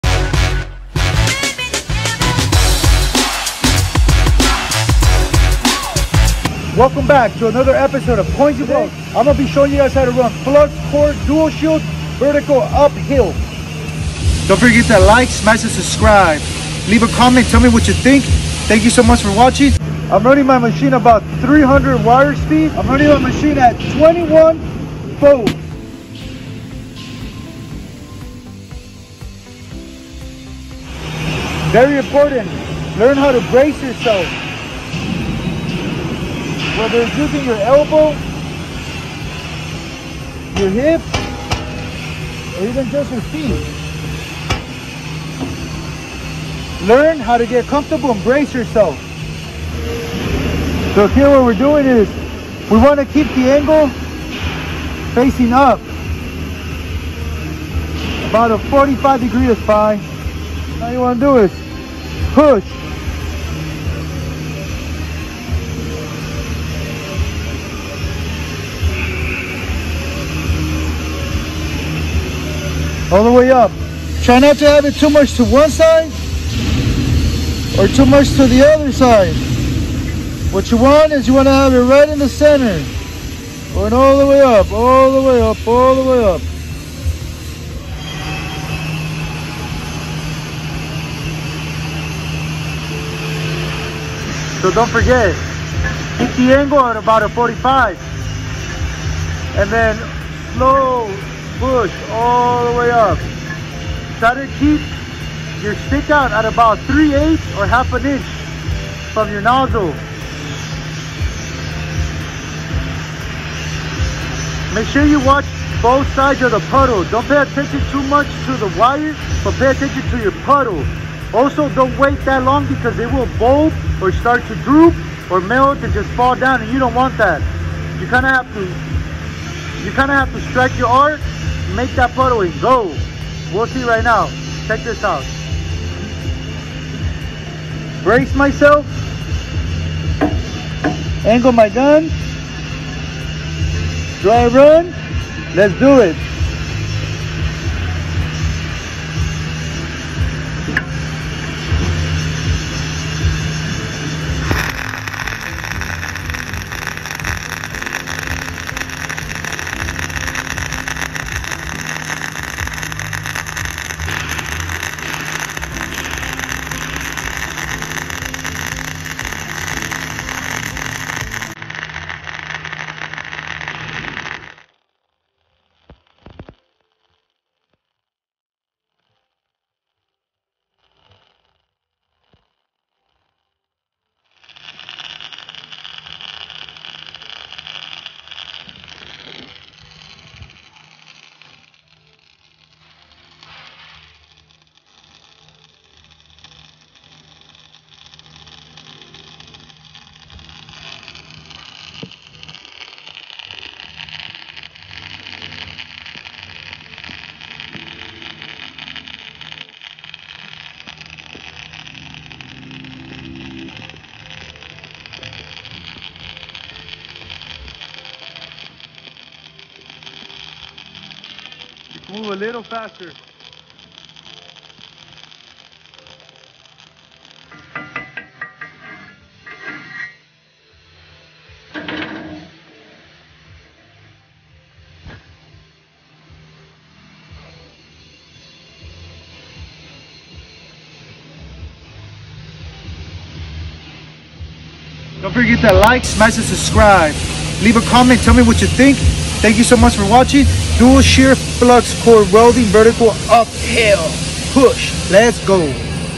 How to weld dual shield fluxcore | FCAW for beginners 3g vertical uphill
Dual shield fluxcore welding for beginners vertical uphill is a great tutorial for the beginner welders trying to improve vertical 3g welding. Keep nozzle angle and speed consistent as best you can . Using .045 fluxcore welding wire . Welding machine is Miller . FCAW is fluxcore arc welding .  im showing some cool fluxcore welding tips and tricks using miller welder machine. even if your a beginner keep welding its ok to try and make mistakes. i show what machine i use what gas and wire for welding. this is a self feeding mechanism . wire feeder welder.